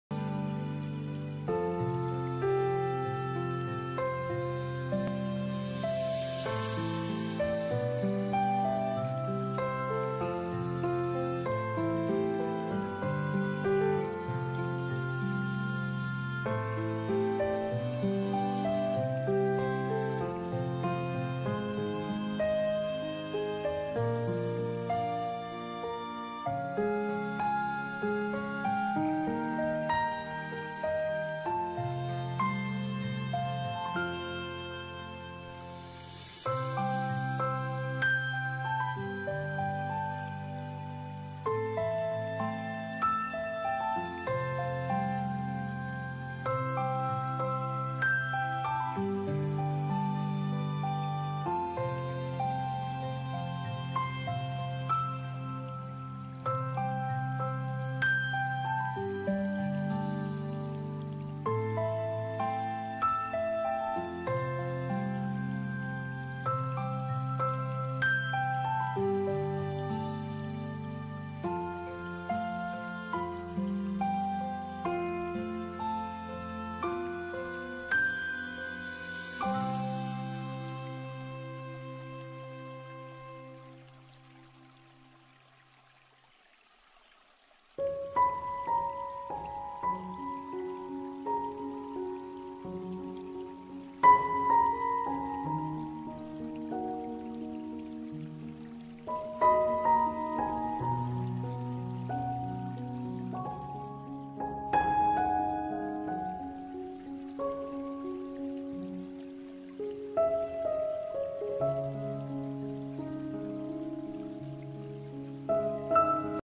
Cencellada
Cencellada:es la formacion de plumas y agujas de hielo suave de color blanco sobre una superficie solida,causada por el congelamiento de goticulas sobrefundidasde un blanco de niebla a temperaturas bajo cero. Las propiedades fisisicas de la Cencellada blanca son muy parecidas a las de la nieve humedapor lo que su apareiencia es similar a la de una nevada. si quereis podeis seguirme por instagram